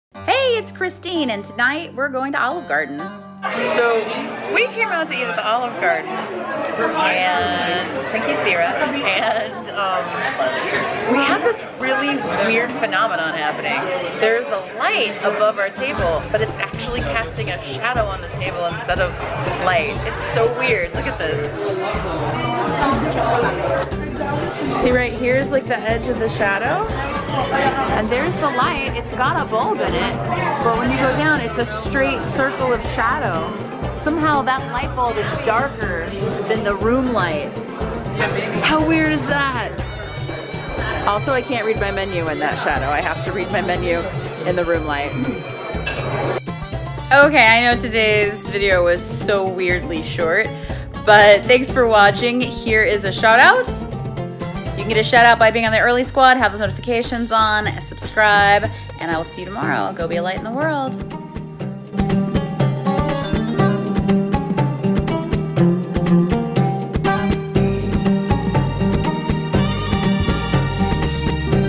Paranormal light at Olive Garden? Daily Vlog 14
Do you think this was a Paranormal light or just a weird natural occurrence?

Thank you Syrah_Gynmast